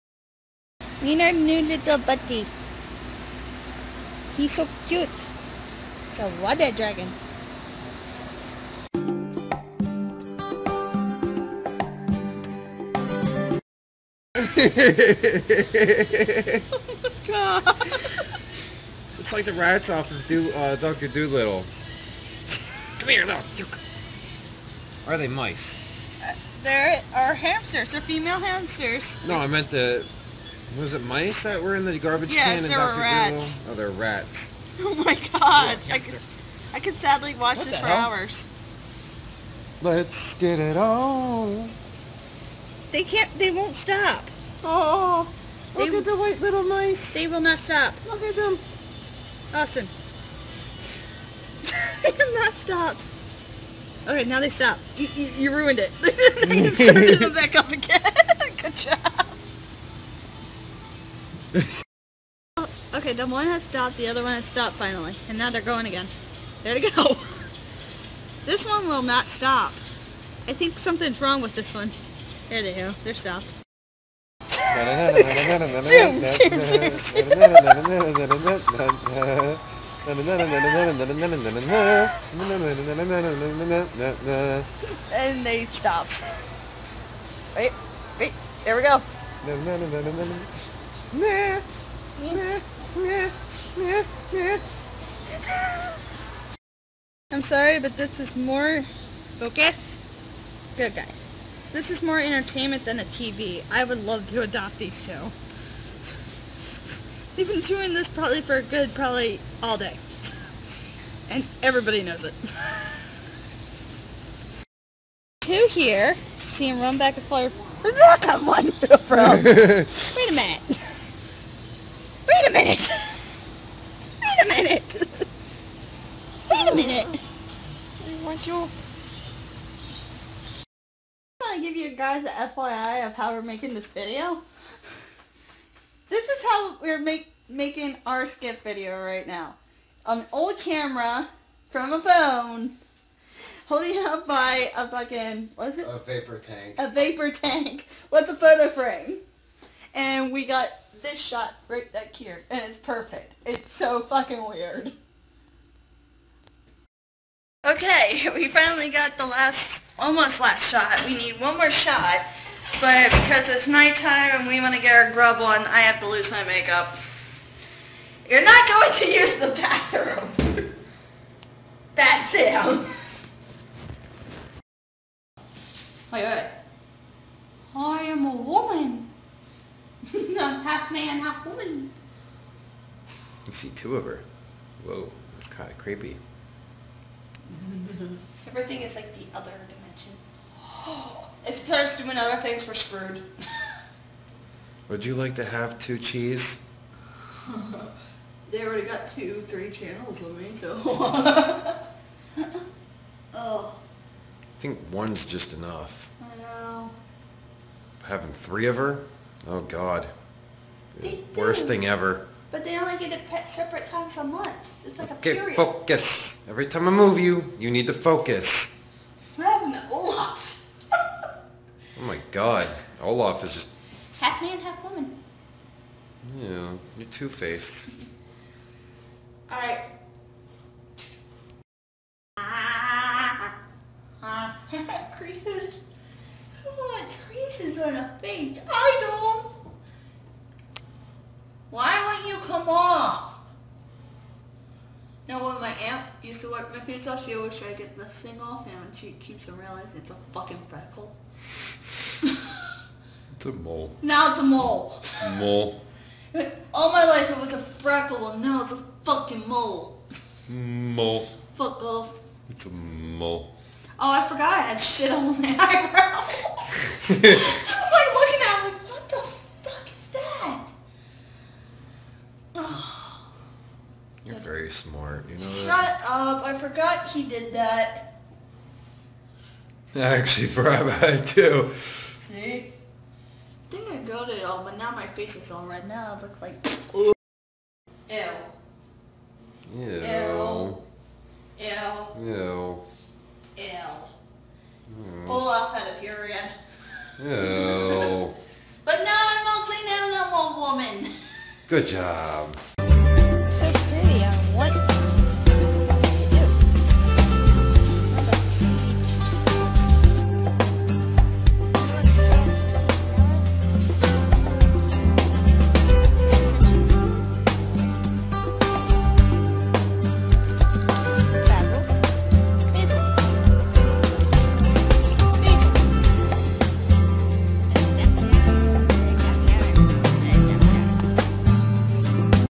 non STOP running HAMSTERS!!! - {5.23.15} (264)
i mean im dressed up as a man and in a pet store, what could go wrong?